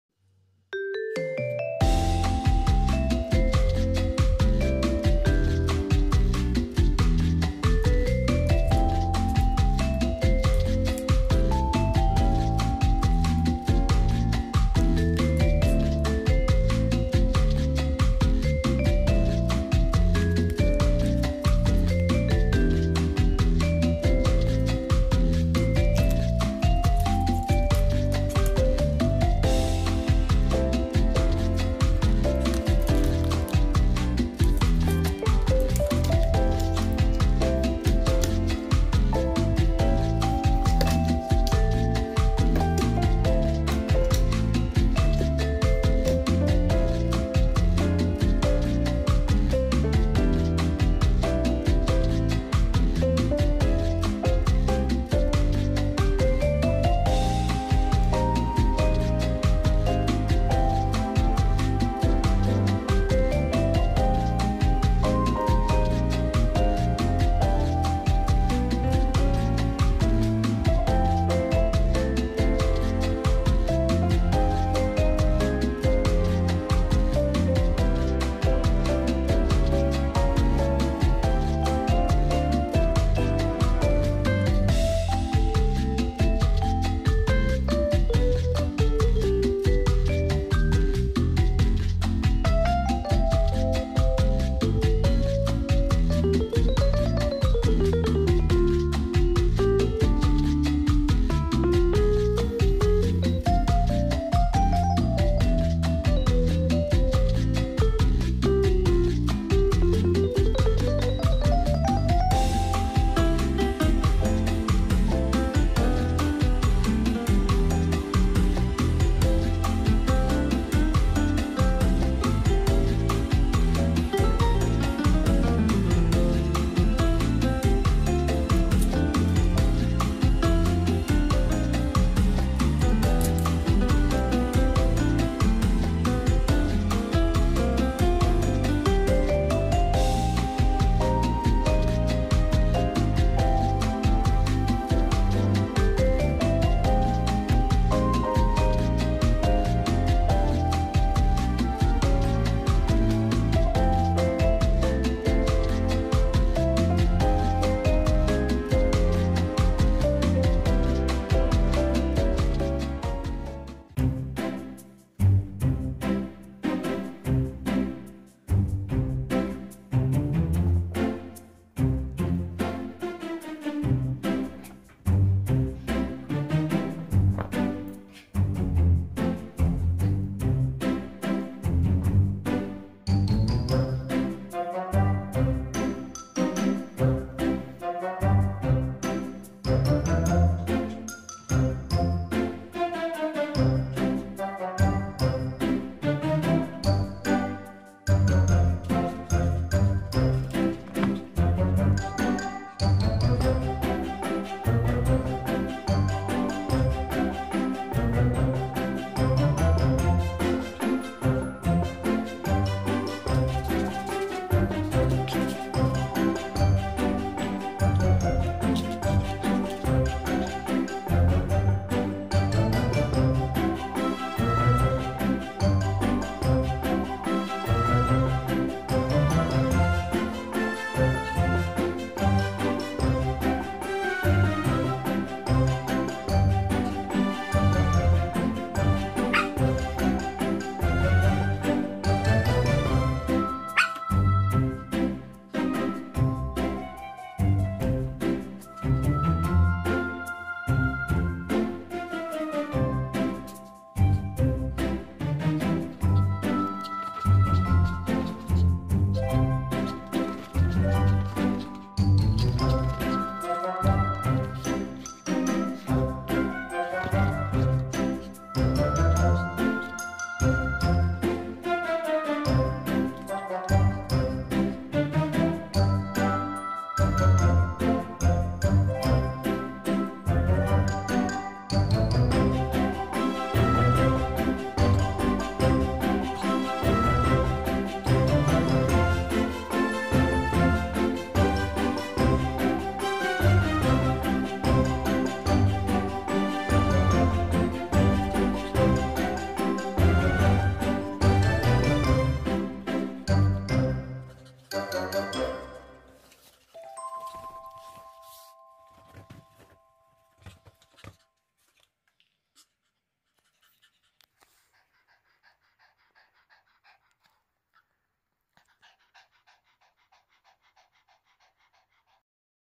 ポップコーンあげてみた（犬用） パピヨン犬
ショップで見つけた犬用お菓子ポップコーン
初めてのお菓子で面白い行動に出ます。
味見しましたが、無味無臭で塩味もありませんでした。